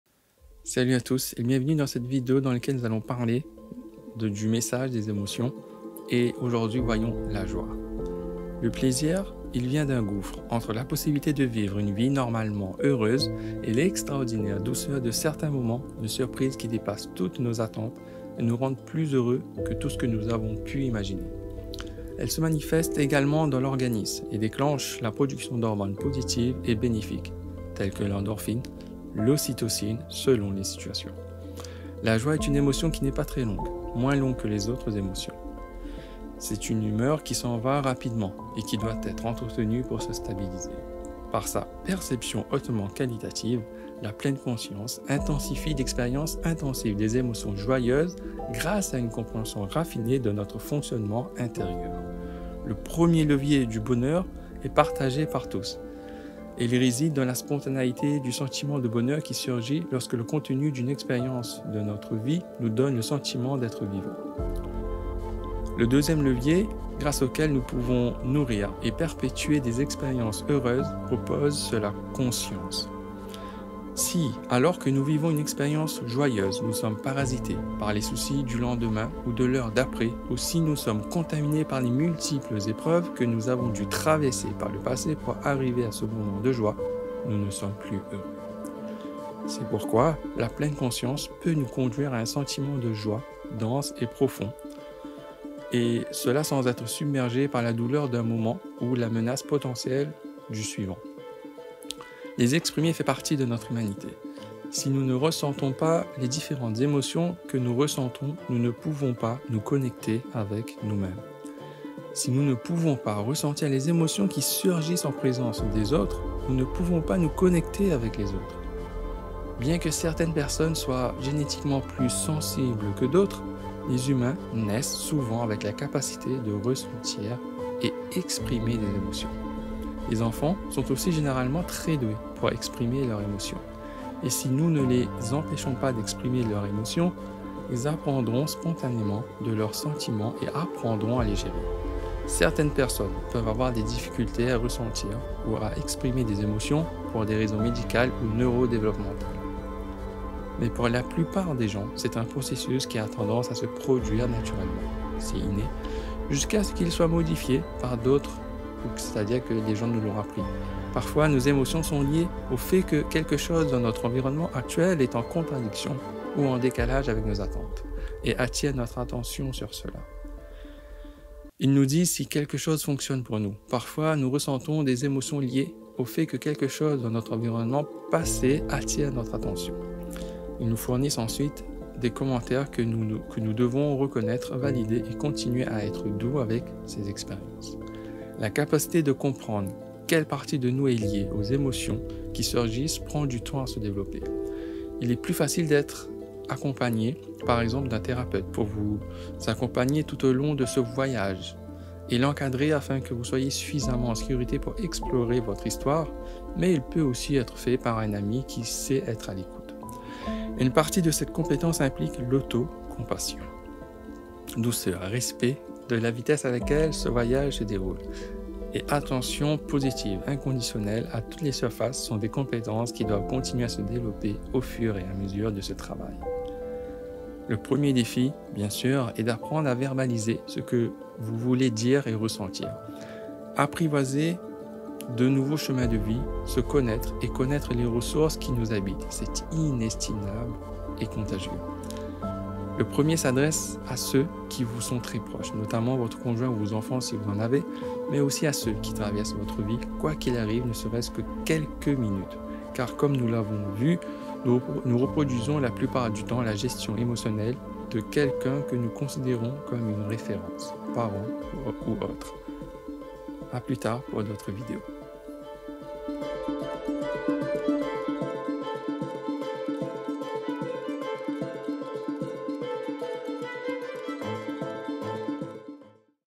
émotions : comprendre le message de la joie 🤩
Bonjour les amis ! Dans cette vidéo, nous allons nous intéresser à l'émotion de la joie. Comment la décrire, pourquoi elle nous touche, et quelles en sont les aspects les plus importants ?

Après cette introduction, nous allons voir quelques moments de joie différents, afin de mieux comprendre le message de la joie.

Enfin, nous allons terminer cette vidéo avec quelques conseils pour vous aider à comprendre la joie autrement. Bon visionnage